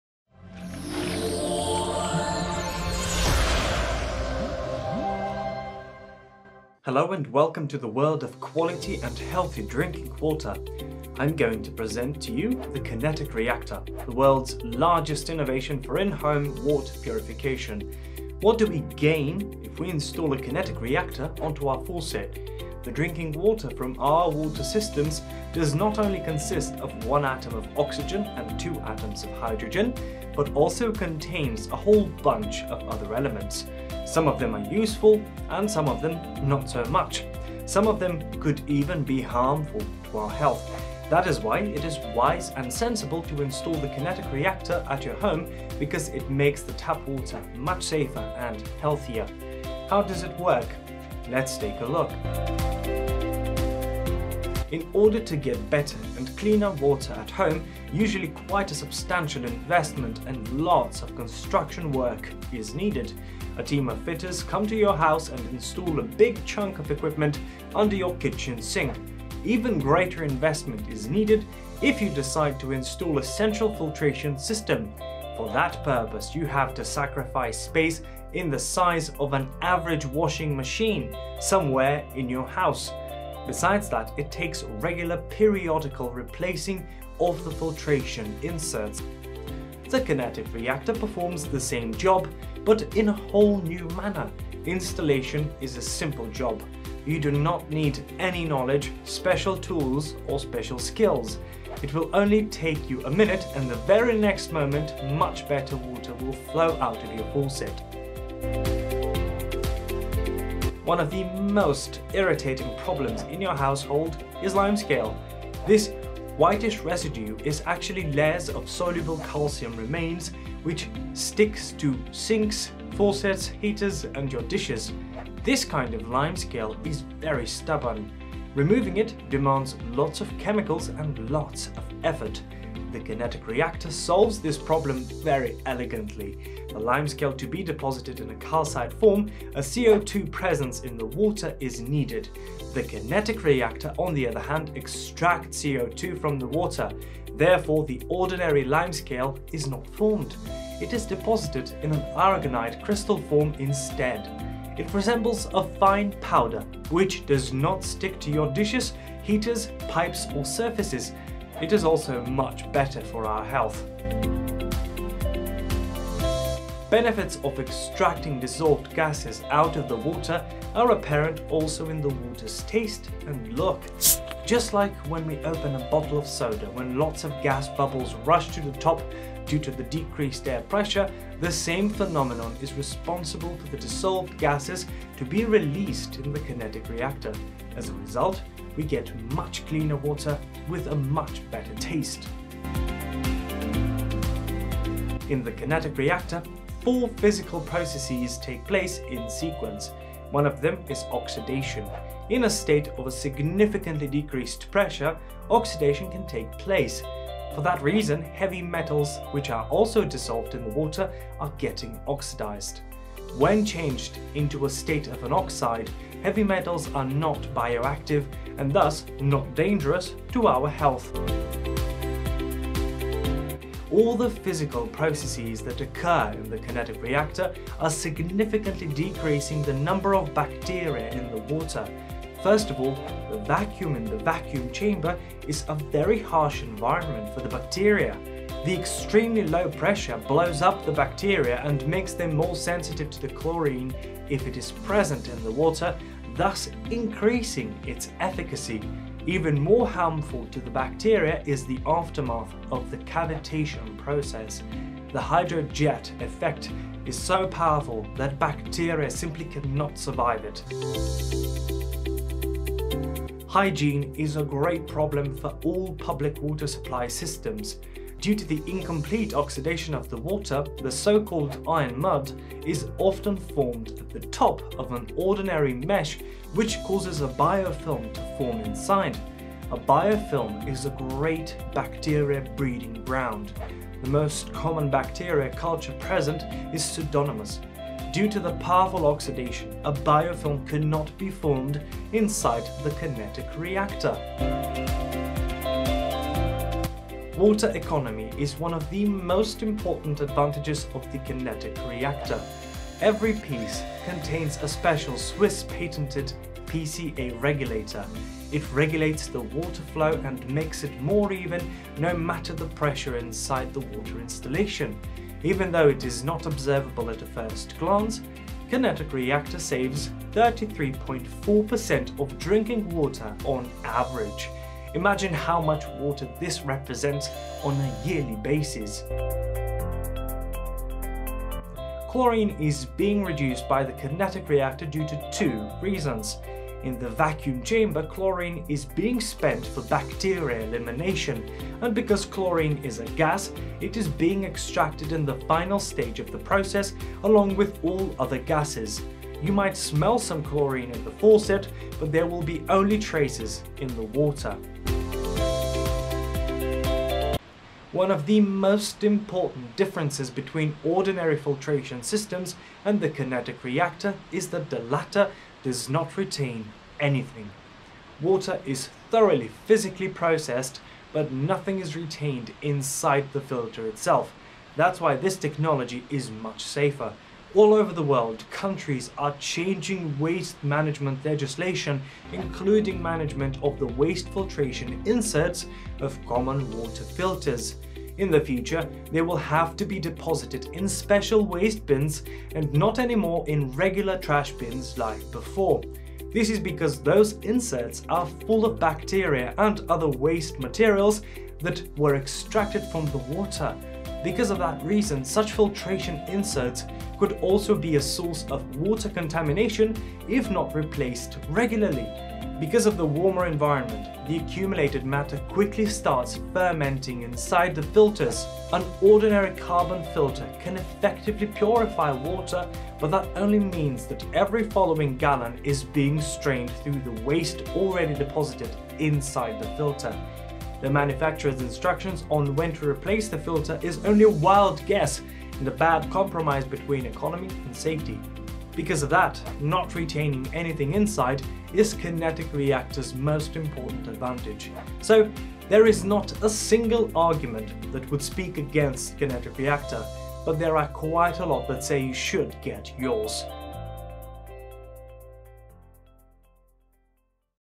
Kinetic Reactor - introduction
Kinetic Reactor is a multi-functional on-tap water treatment device for domestic usage that uses an existing water stream for its efficient operation, which does not require additional energy and chemicals. In the heart of Kinetic Reactor chamber, hydrodynamic cavitation, oxidation processes and mechanical forces are used for water treatment.